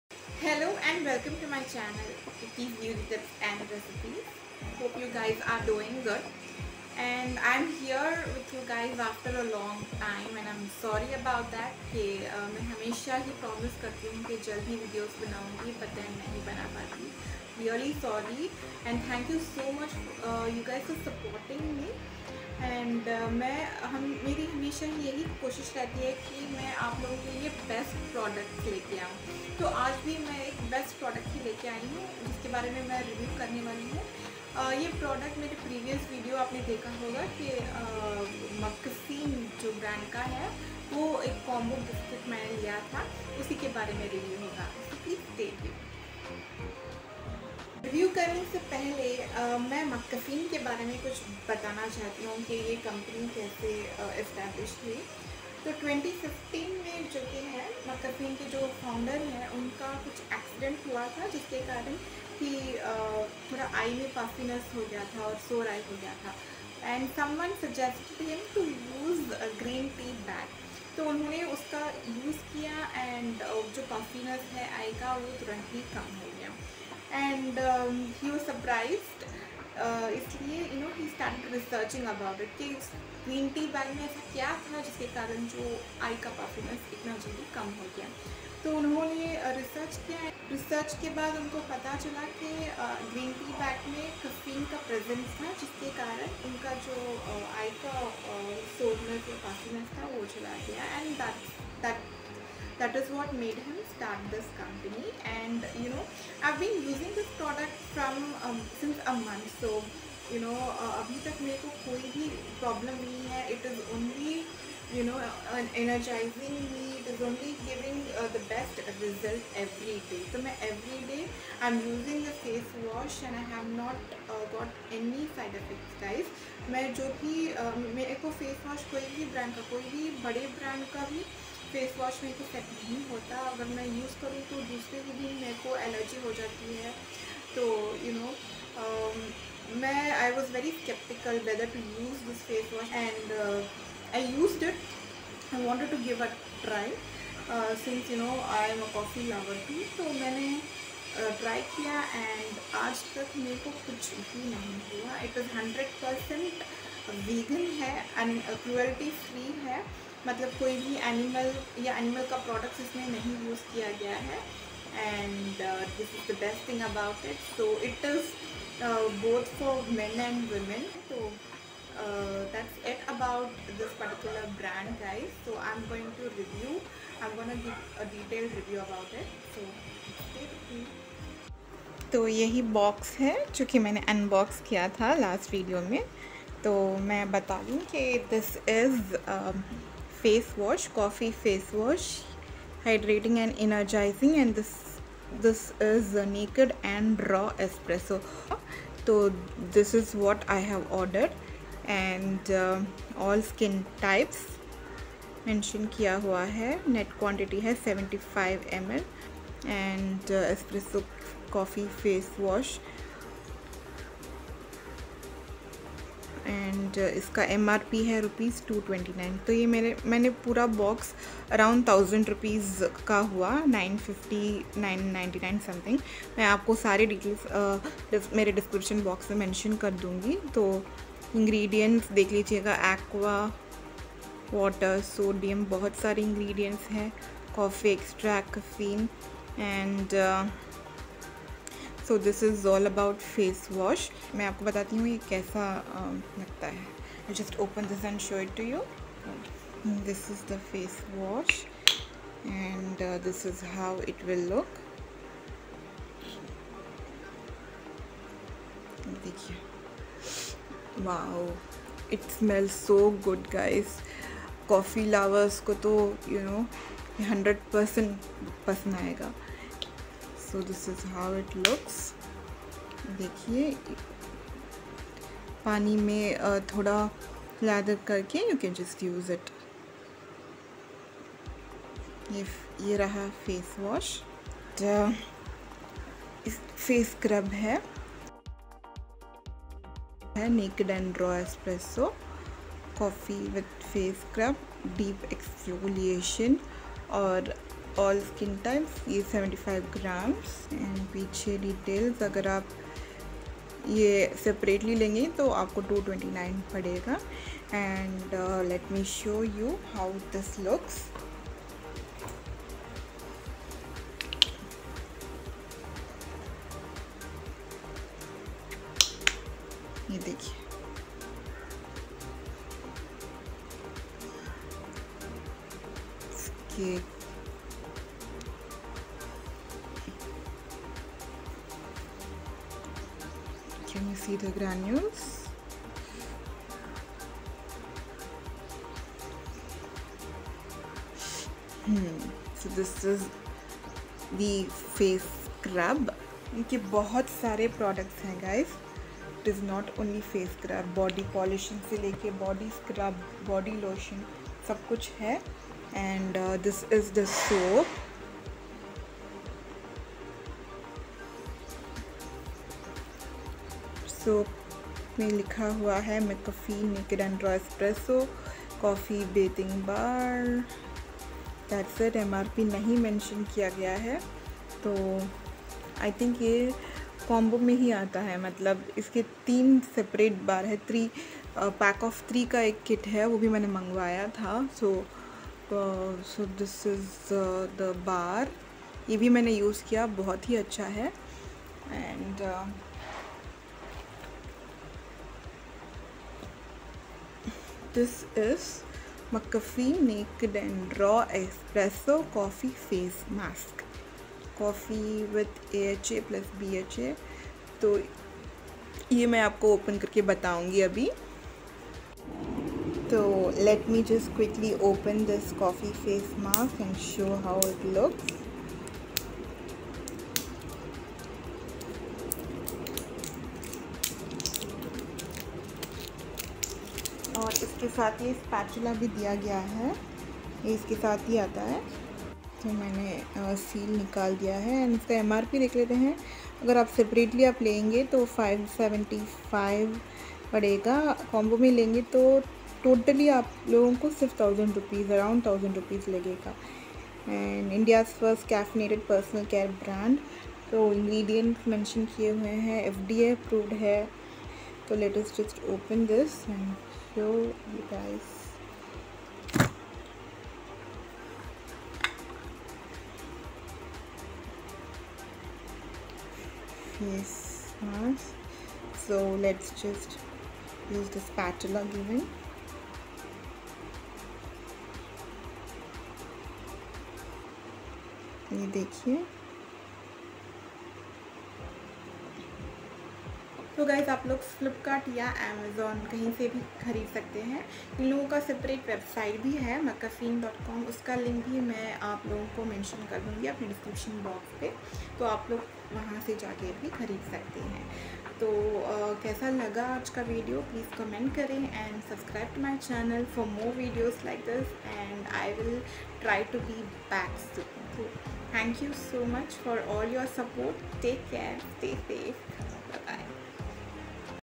mCaffeine Review || mCaffeinenakedandrawespresso
Hello Everyone! I am back with a detailed review about mCaffeine brand and products. I had ordered a combo kit which has Face wash, Face scrub, Face mask and Bathing bar. I have tried and have given a genuine review about it in this video. So please don't miss this video.